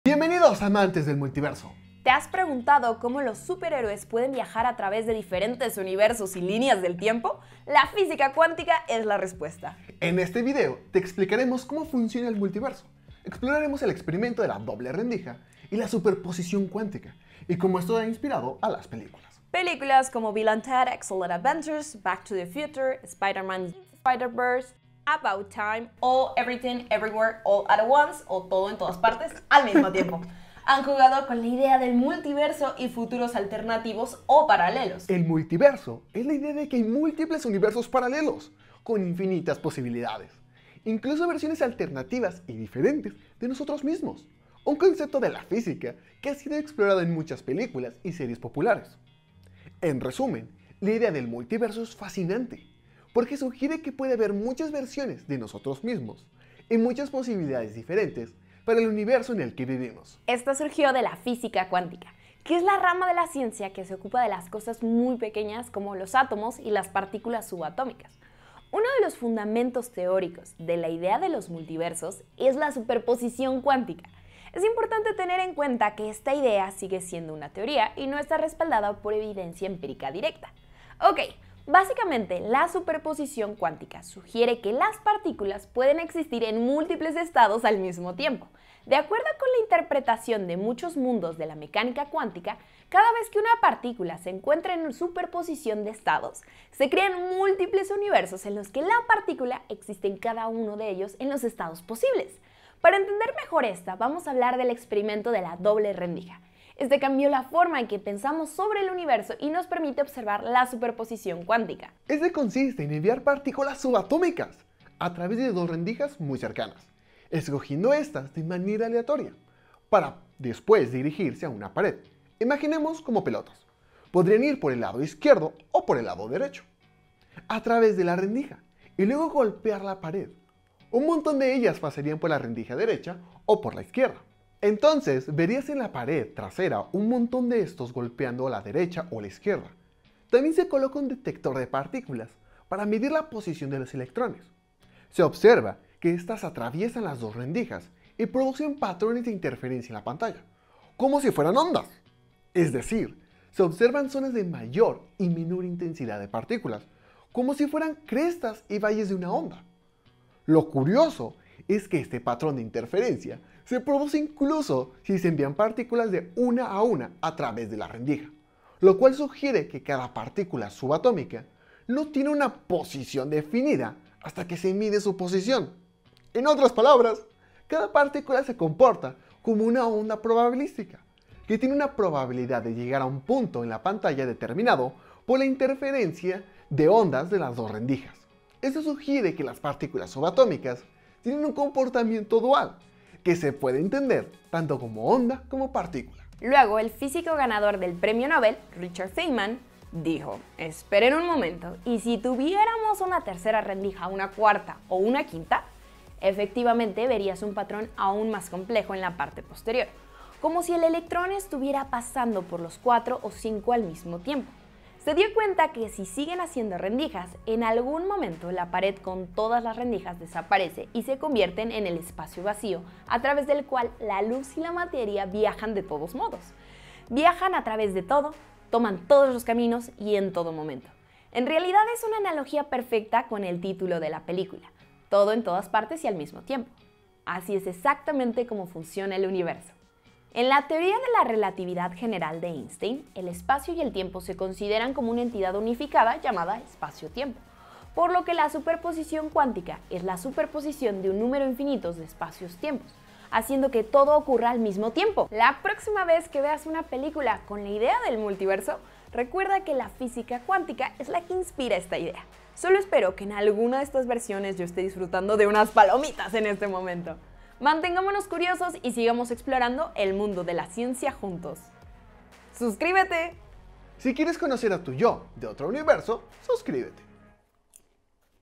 El Experimento de la Doble Rendija: la Física Cuántica y Multiversos 🤔 - MoonMakers
¿Te has preguntado alguna vez si hay otras versiones de ti mismo en realidades alternativas?
Hoy explicamos cómo funciona el multiverso, exploramos el experimento de la doble rendija y la superposición cuántica y cómo esto inspira a las películas, como la ganadora del Oscar 'Todo en Todas Partes al Mismo Tiempo'.